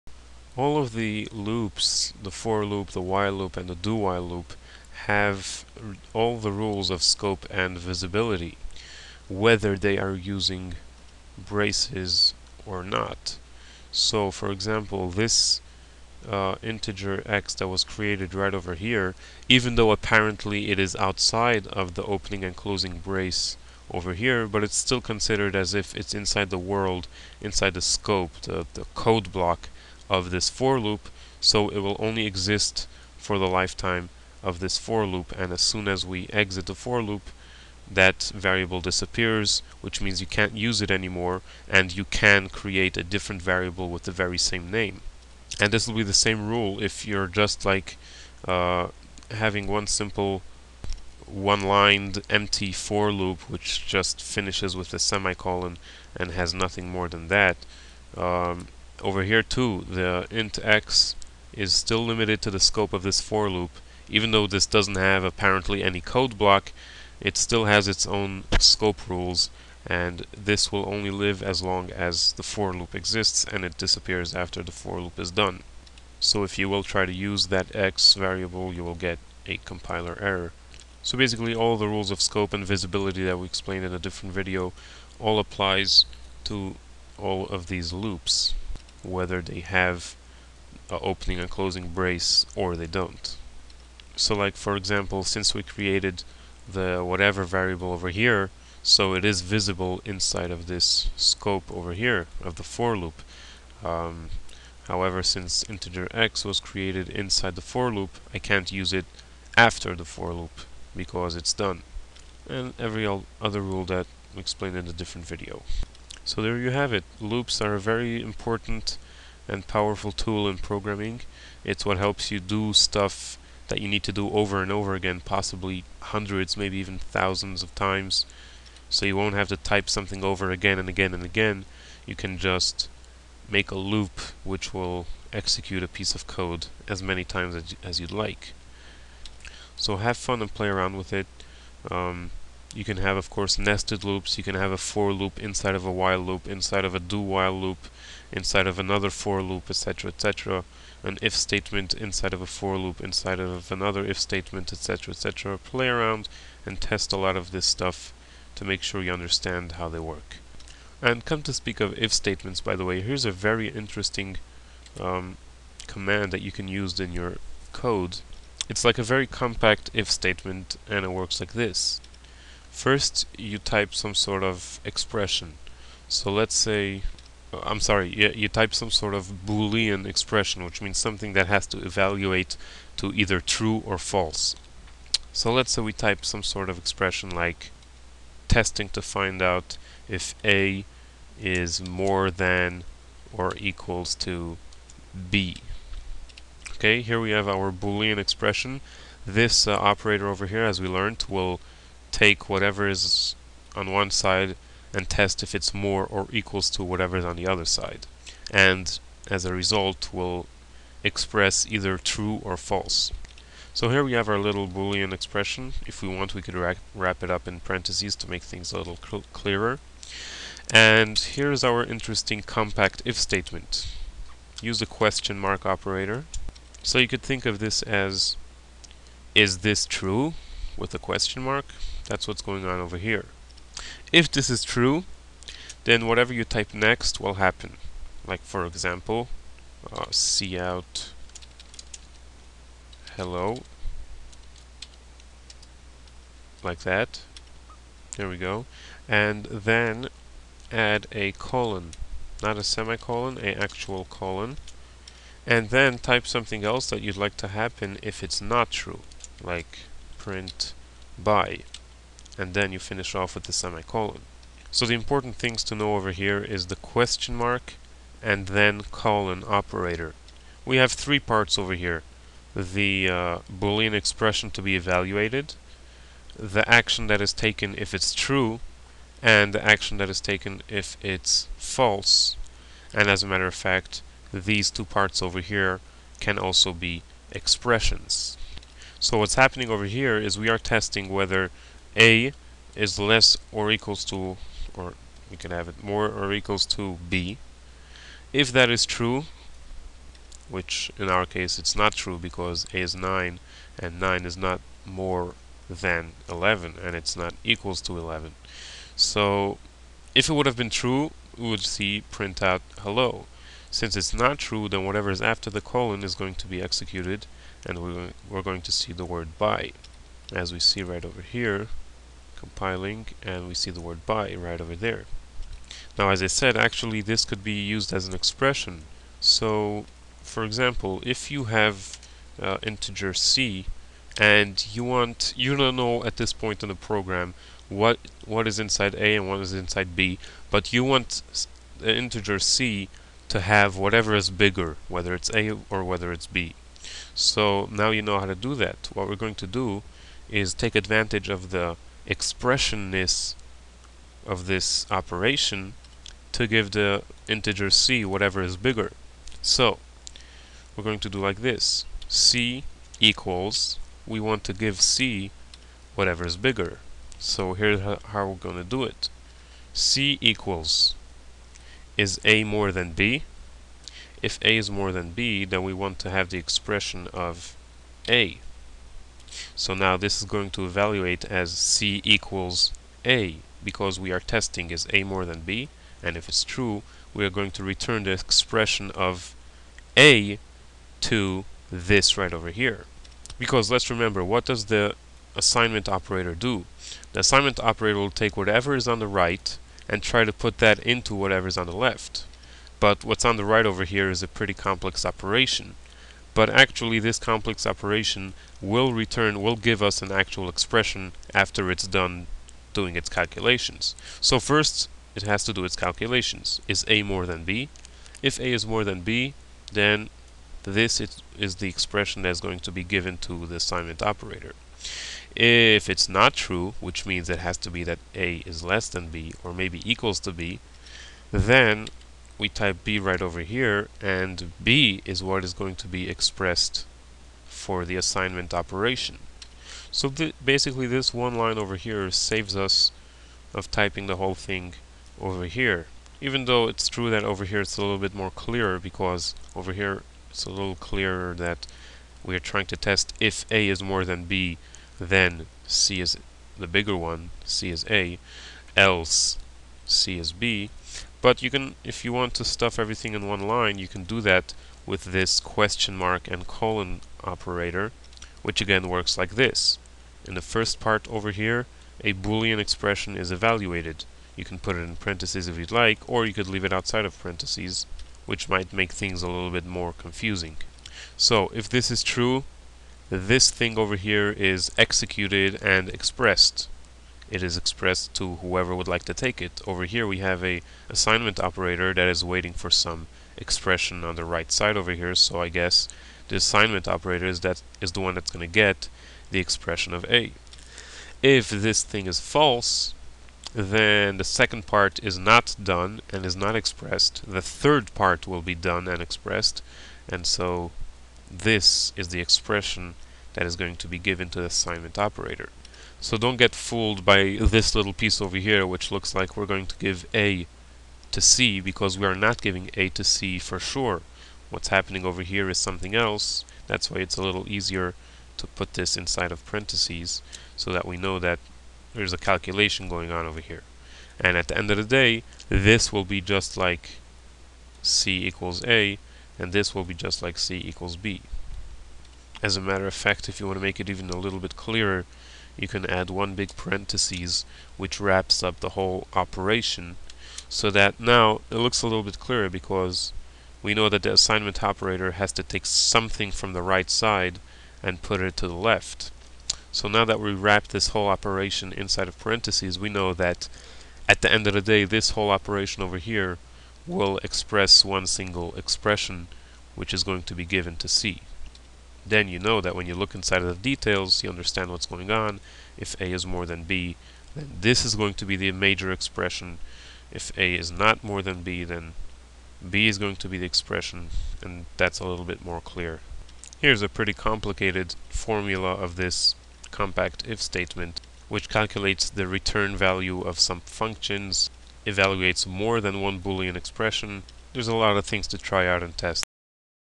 C++ Tutorial (25) - Absolute n00b spoonfeed
Video tutorials about programming in the C++ programming language.

Part 25:
* Scope/Visibility in loops
* Conditional operator

Pardon my imperfect english.

If you have any C++ questions, please comment.